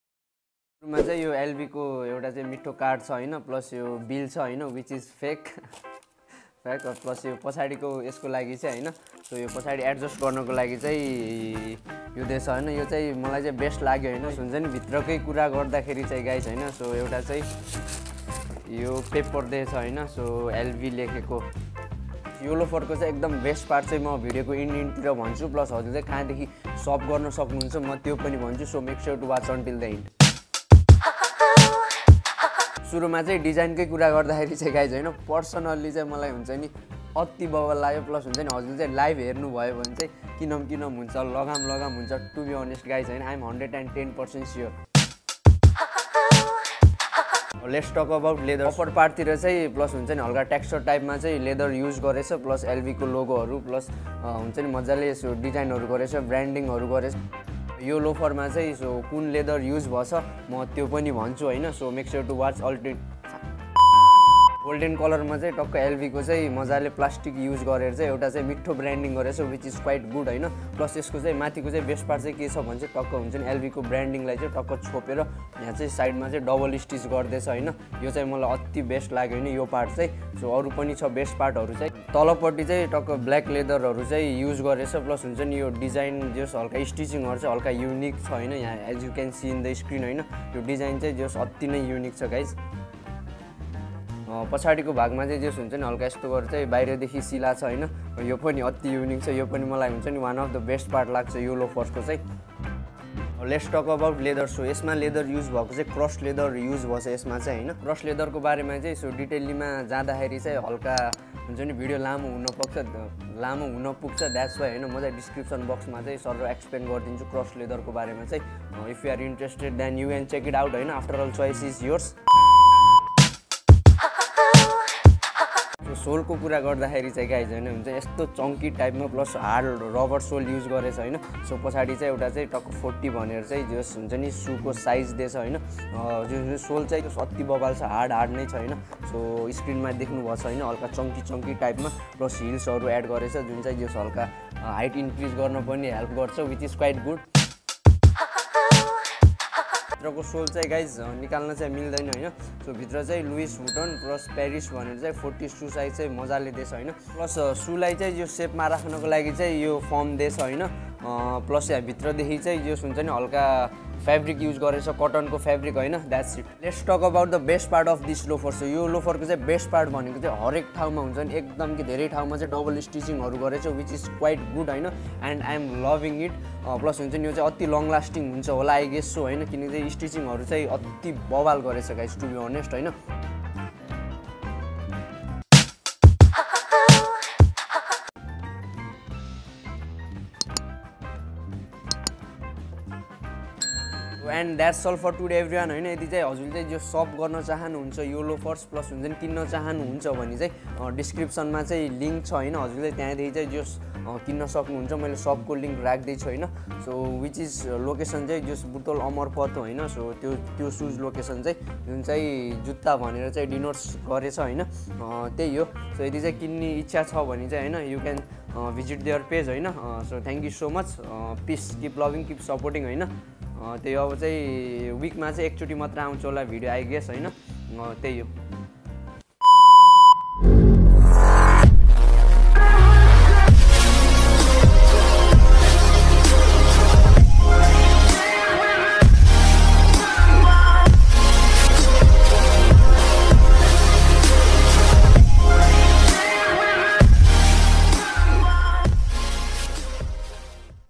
Unboxing LOUIS VUTTION Loafer's || MAJOR LOAFERS
What's going on guys its been along day isn't it I really missed you guys hears the brand new video in your YouTube time line i hope you will like it …

let me remind you its not a original its a high copy loafers which is available in Nepal if you are planning to shop then link is available scroll down .
location; butwal amarpat, near haatbazer